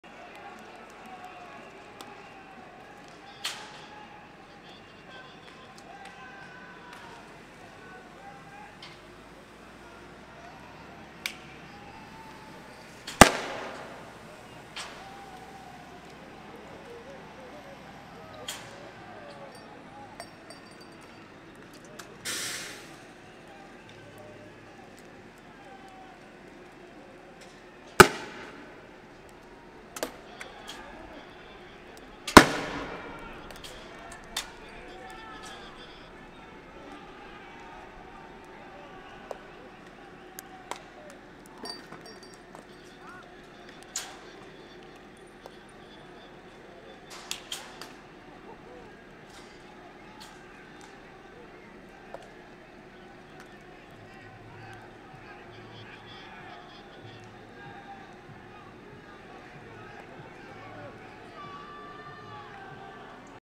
Represión, 20 de diciembre
video en el que se muestra accionar de carabineros y el uso indiscriminado de bombas lacrimógenas